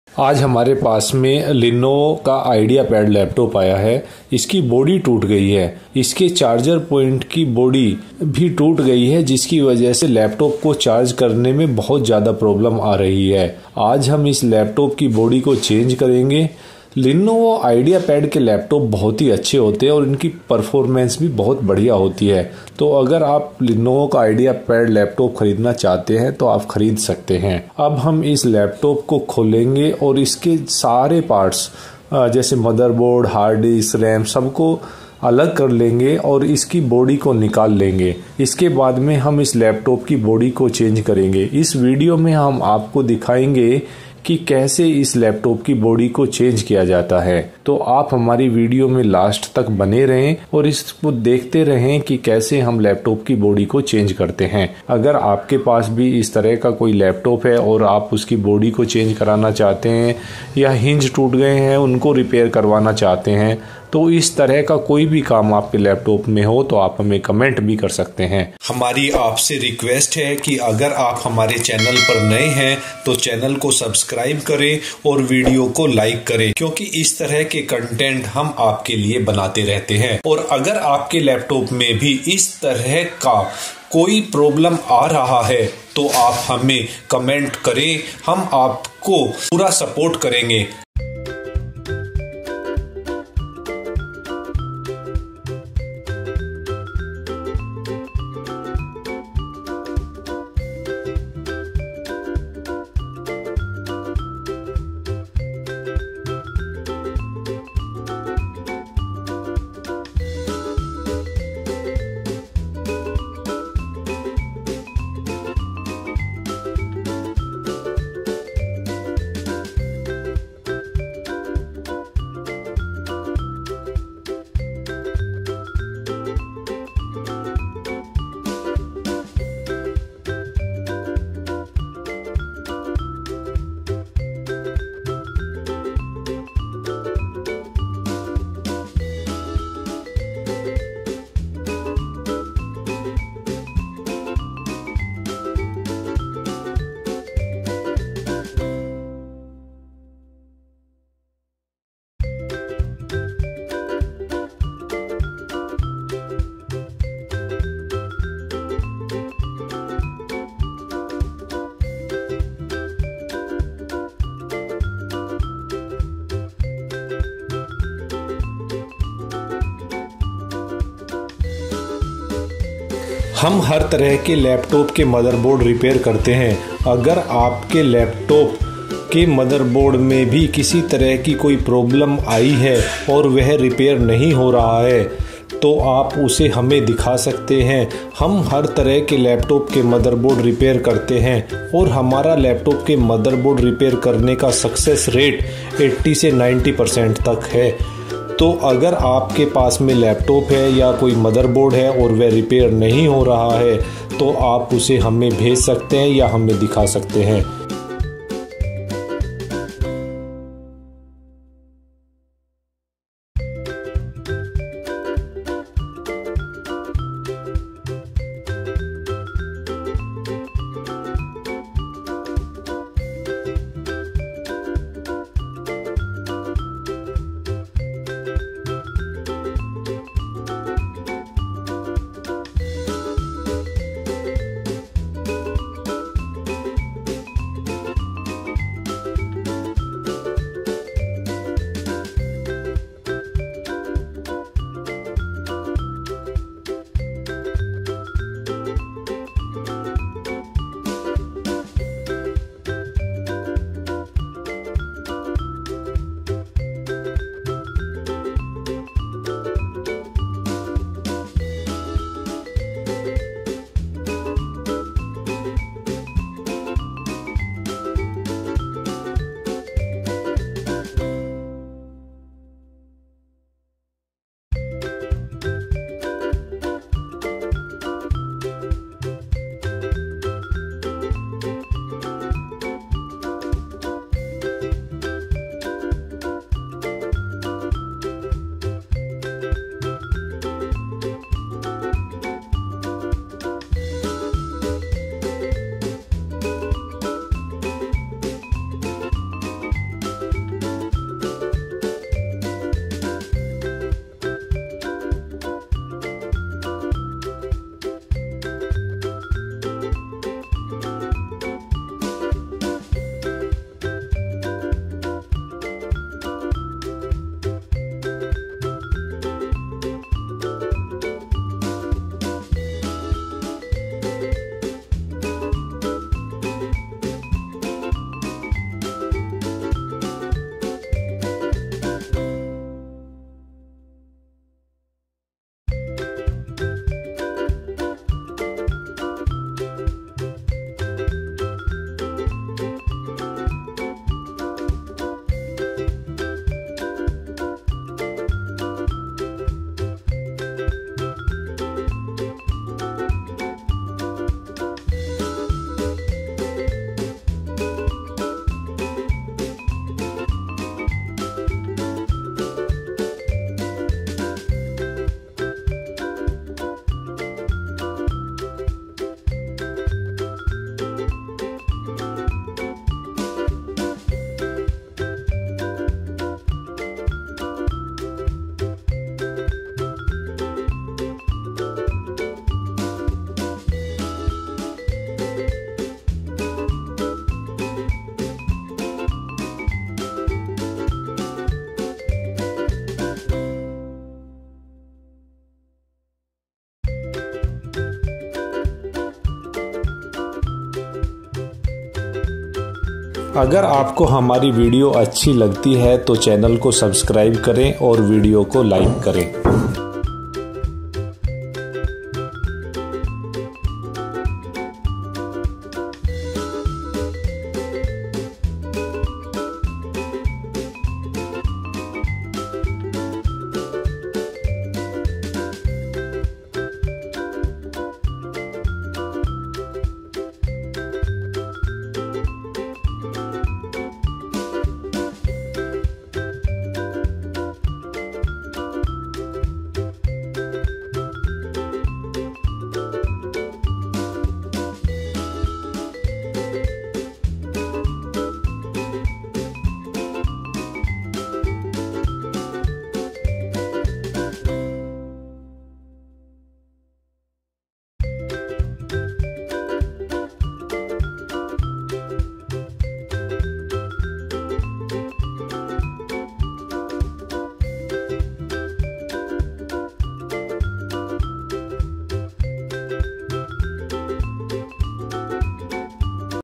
Lenovo Laptop Body Panel | Lenovo IdeaPad Bottom Base Cover Case Replacement
If you need to replace the bottom base cover case on your Lenovo IdeaPad laptop, you can follow these steps:

Turn off your laptop and unplug it from the power source.
Flip the laptop over so that the bottom is facing up.
Locate the screws that hold the bottom base cover in place. They are usually marked with small icons that look like circles with lines coming out of them.
Use a small Phillips head screwdriver to remove the screws.
Gently pry off the bottom base cover. You may need to use a plastic spudger or similar tool to help loosen it if it is stuck in place.
Once the cover is removed, you can disconnect any cables or connectors that are attached to it. Make note of where they are located so that you can reconnect them later.
Take the new bottom base cover and align it with the screw holes on the bottom of the laptop. Make sure any cables or connectors are connected before you attach the cover.
Replace the screws and tighten them carefully. Do not overtighten, as this can damage the threads or the screw holes.
Once the bottom base cover is securely attached, you can turn the laptop back on and check that everything is working properly.